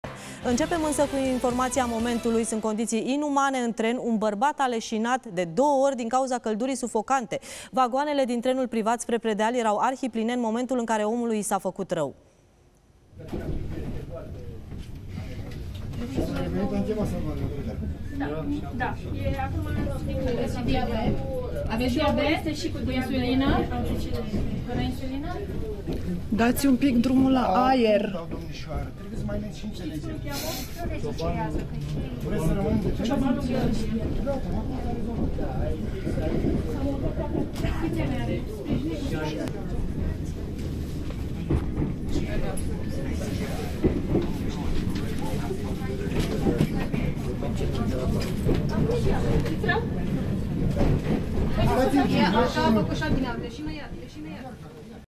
Condiții inumane într-un tren | Un bărbat a leșinat de două ori din cauza căldurii
Un bărbat în vârstă de 69 de ani a leșinat de doua ori în tren, din cauza căldurii sufocante